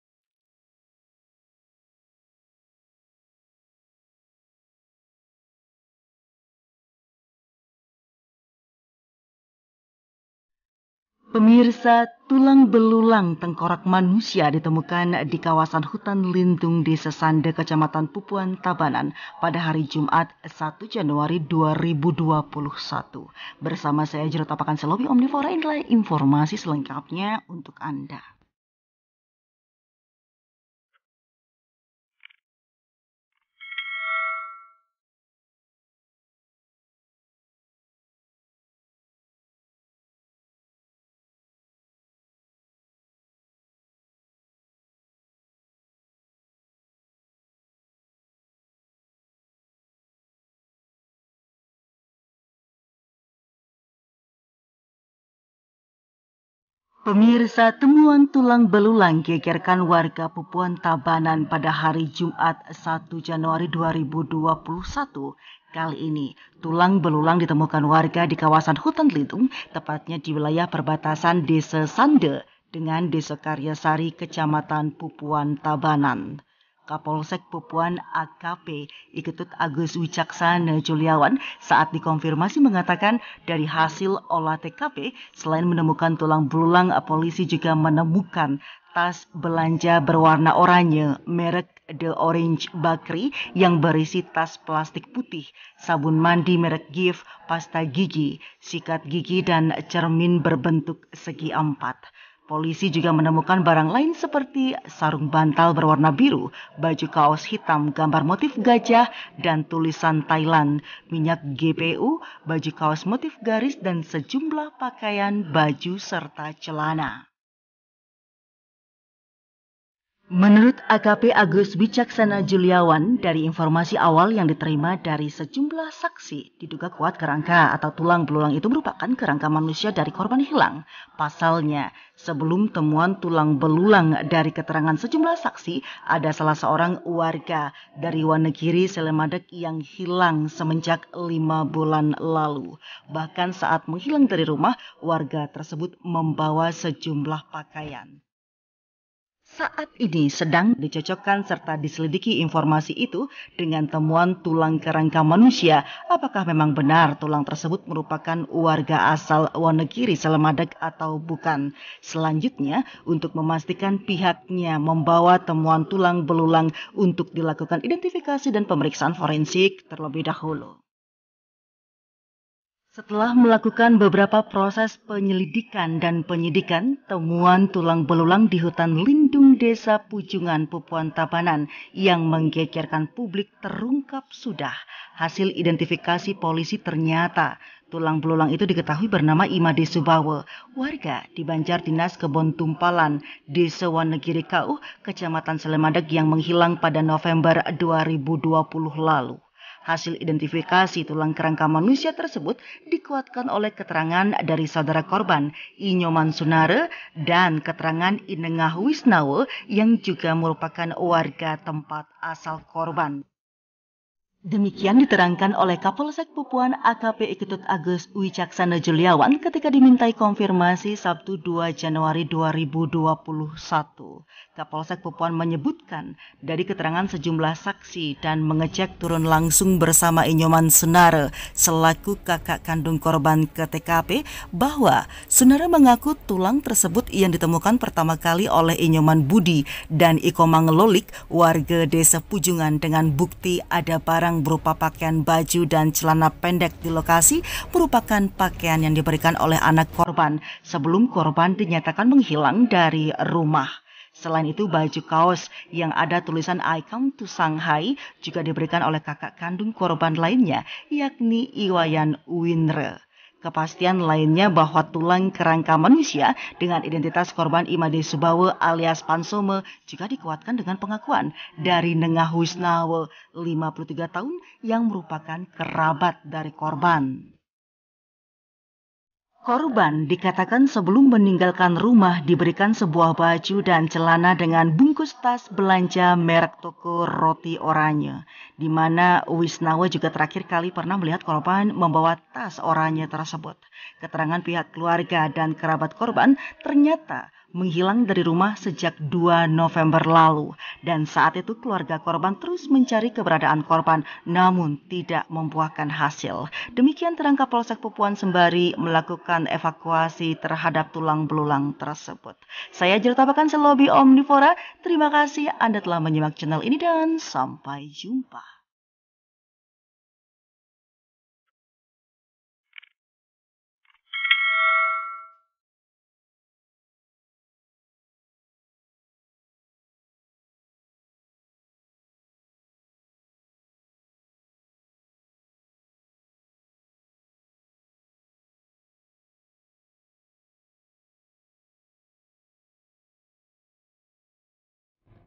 TEMUAN TULANG BELULANG DI HUTAN PUJUNGAN DESA SANDA TABANAN GEGERKAN WARGA PUPUAN!
Temuan tulang belulang di hutan lindung Desa Pujungan, Pupuan, Tabanan yang mengegerkan publik terungkap sudah.

Hasil polisi ternyata tulang belulang diketahui bernama I Made Subawa, warga di Banjar Dinas Kebon Tumpalan, Desa Wanagiri Kauh, Kecamatan Selemadeg yang menghilang pada November 2020 lalu.

"Hasil indentifikasi kerangka kerangka manusia tersebut dikuatkan oleh keterangan dari saudara korban I Nyoman Sunara dan keterangan I Nengah Wisnawa yang juga merupakan warga tempat asal korban," terang Kapolsek Pupuan AKP I Ketut Agus Wicaksana Julyawan, Sabtu 2 Januari 2020.